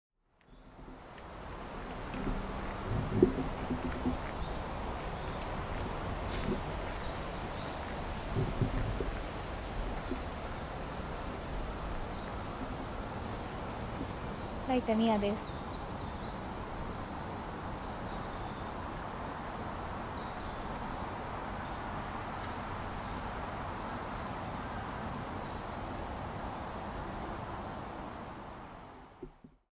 美女暦14年7月号★来田みや★ Japanese Kimono Girls
夏の美『浴衣美女』。

日本において夏の風物詩は、風鈴の音、花火、蛍の光、田舎に帰省した時の隣のお姉さん、などなど色々あるけれど、『浴衣を纏った美女』に敵う日本の伝統美があるだろうか。

いや、ない！

布一枚で、ここまで妖艶にも可愛くも魅せることが出来る浴衣の神秘は何なんだろう。
今月の美女暦は浴衣美女を特集します。

いつまでも褪せることの無い、最高の夏の美『浴衣美女』をお楽しみください。

美女暦